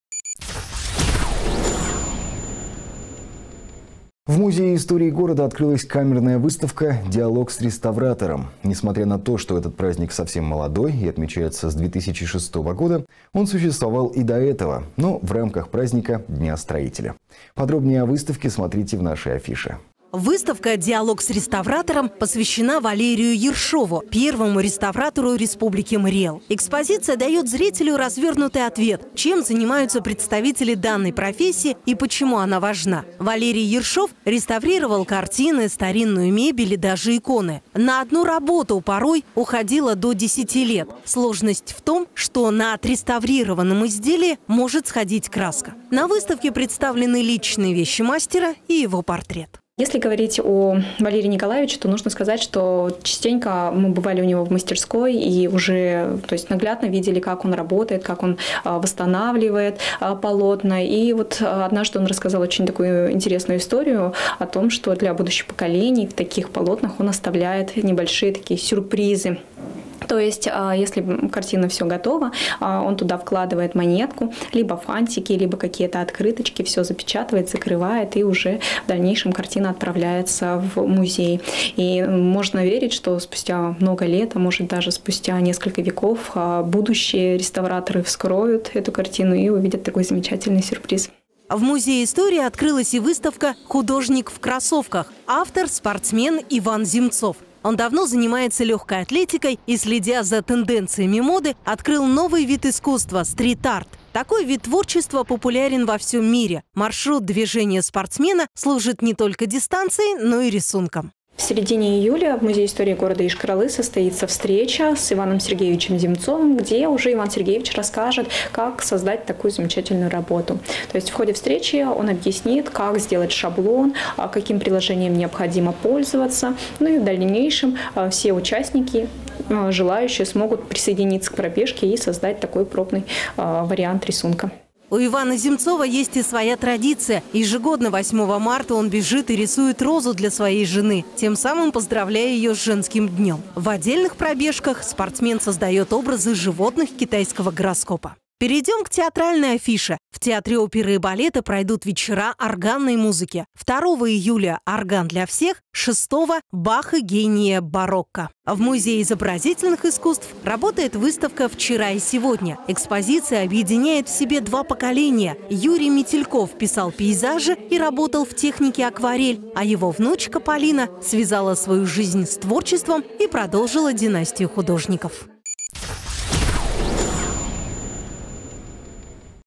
Рубрика «Культурная афиша»
В музее истории города открылась камерная выставка «Диалог с реставратором». Несмотря на то, что этот праздник совсем молодой и отмечается с 2006 года, он существовал и до этого, но в рамках праздника Дня строителя. Подробнее о выставке – смотрите в еженедельной рубрике «Культурная афиша».